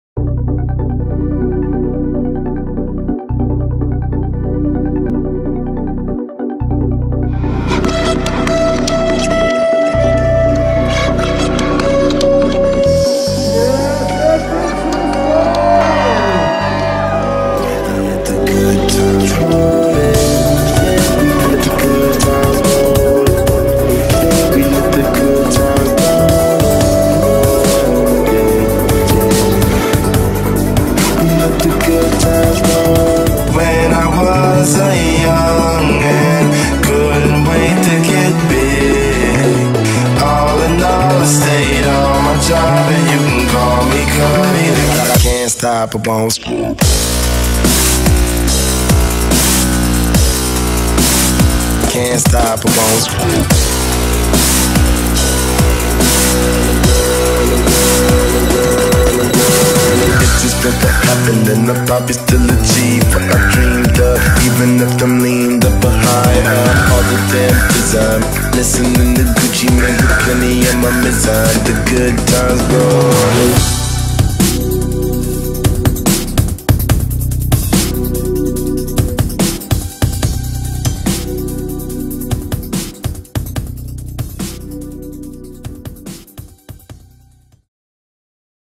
Steve Aoki afterparty @ Madrone Studios SF
Steve Aoki teamed up with Madrone Studios to present a night of immersive dance music in San Francisco. The event was held after Madrone launched Microsoft's Azure operating system at their interactive event space.